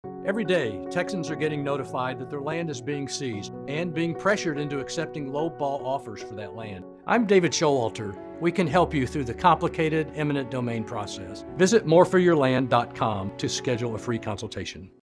Showalter Law Firm - Eminent Domain Attorneys
Showalter Law Firm - Free consultation and we don’t get paid unless we get you more money for your land. We have helped thousands of Texans get more money for their land. We can help you through the complicated Eminent Domain process. We will fight to you get a fair and just settlement for your land.